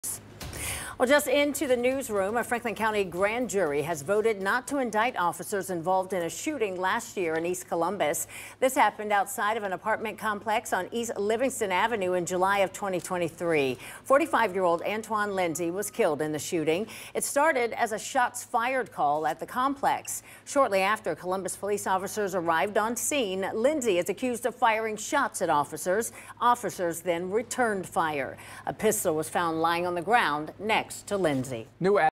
Grandy jury declines to indict officers involved in fatal shooting at Columbus apartment complex
The incident happened on July 8, 2023, and resulted in the death of 45-year-old Antwan Lindsey, who was shot 15 times.